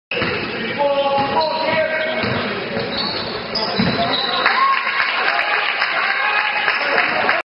BU: Bremond McClinton Off the Glass
BU: Bremond McClinton Off the Glass - High School Boys Basketball: Episcopal vs. Bullis 1/29/2009